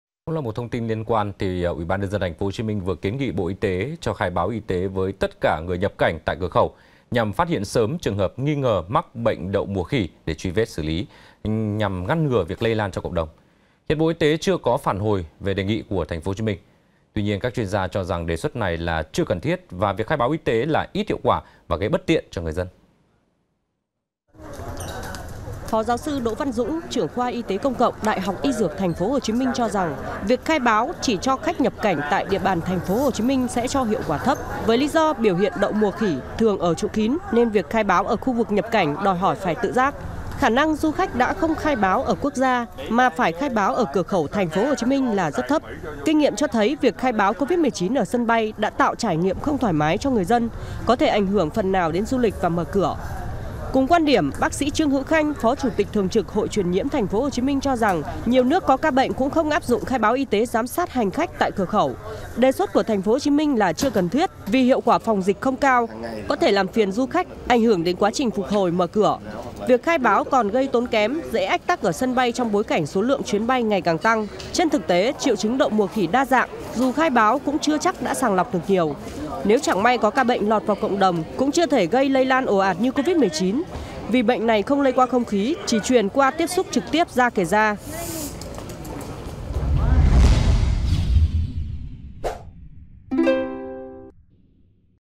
Chưa cần thiết khai báo Y Tế để ngăn Đậu Mùa Khỉ | VTC14
VTC14 |UBND TP.HCM vừa kiến nghị Bộ Y tế cho khai báo y tế với tất cả người nhập cảnh tại cửa khẩu, nhằm phát hiện sớm trường hợp nghi ngờ mắc bệnh đậu mùa khỉ để truy vết, xử lý, ngăn lây lan trong cộng đồng. Hiện, Bộ Y tế chưa có phản hồi về đề nghị của TP.HCM. Tuy nhiên, các chuyên gia cho rằng đề xuất này là chưa cần thiết và việc khai báo y tế là ít hiệu quả, gây bất tiện cho người dân.